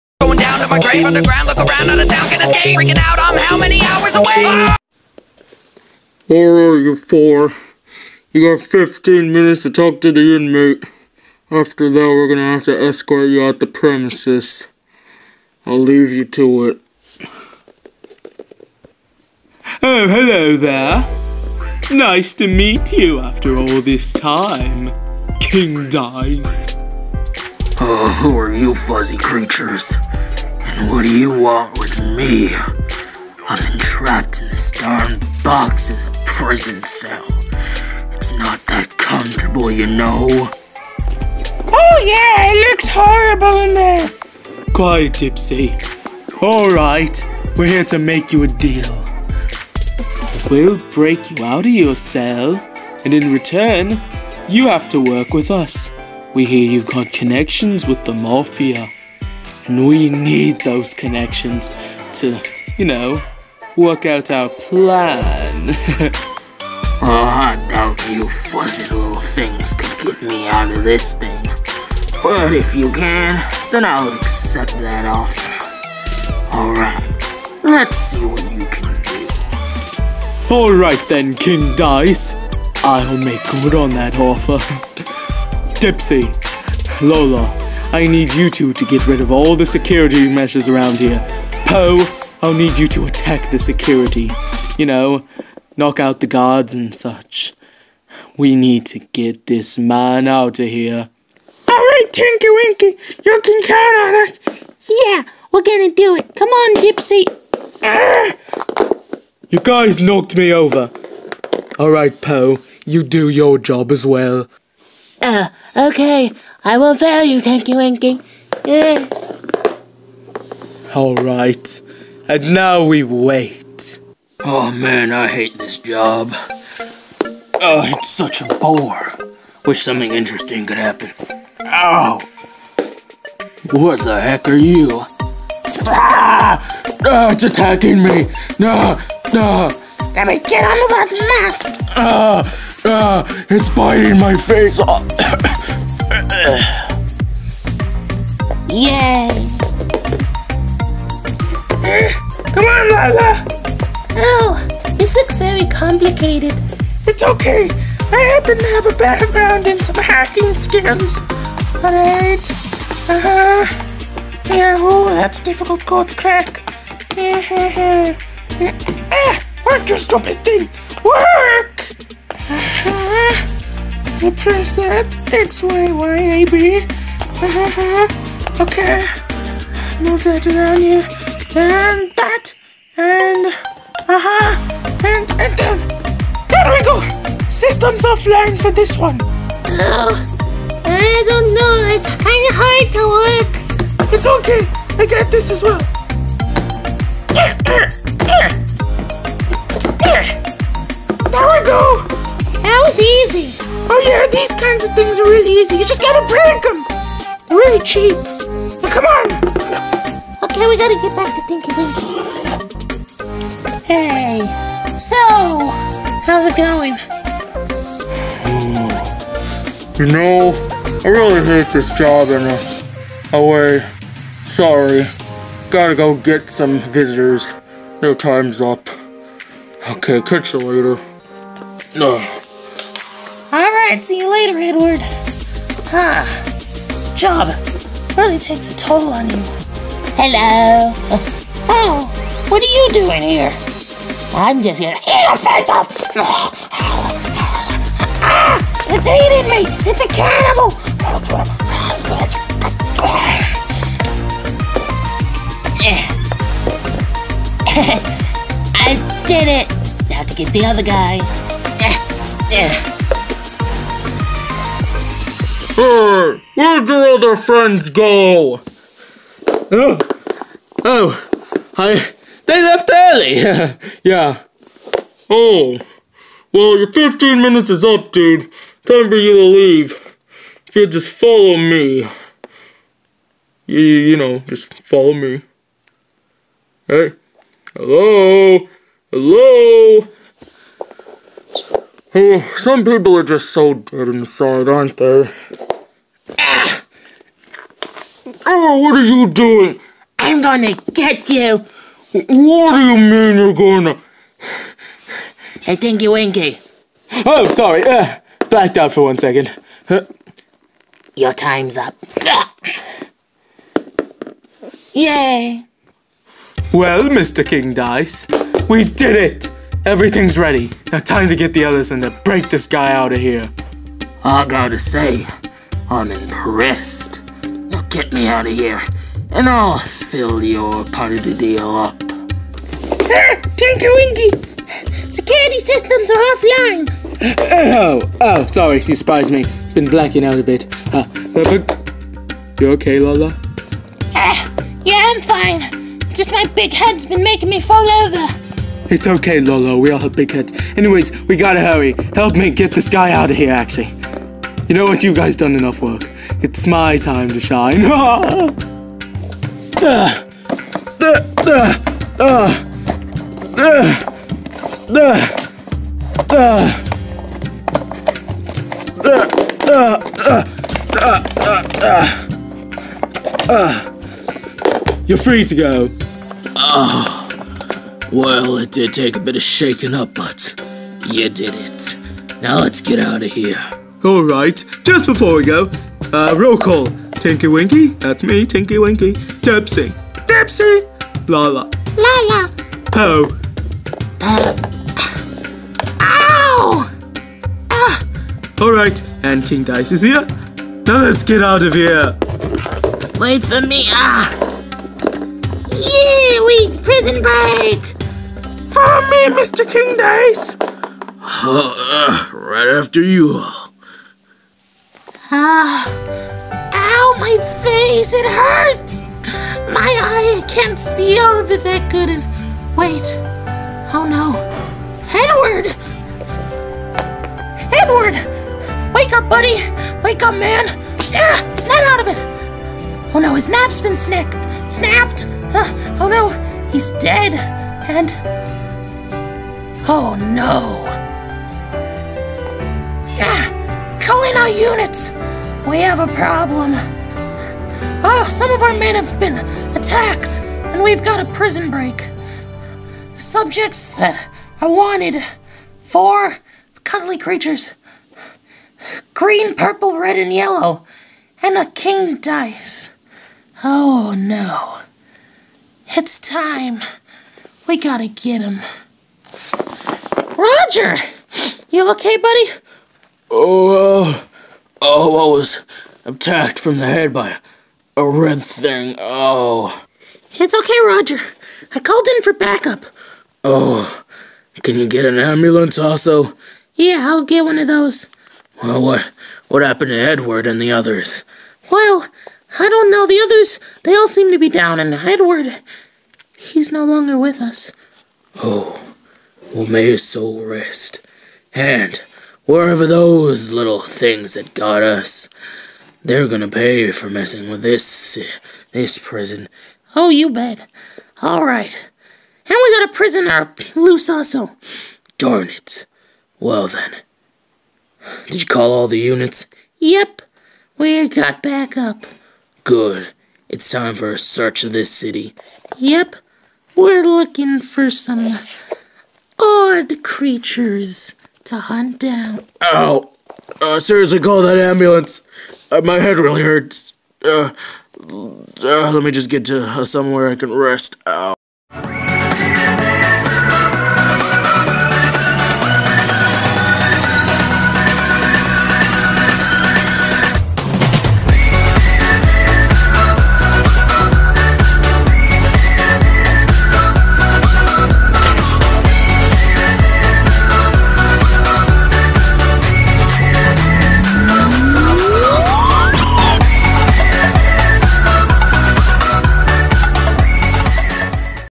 Teletubby Takeover: Episode 2
The Teletubbies find out that King Dice has connections with the mafia, which leads them to a prison breakout mission!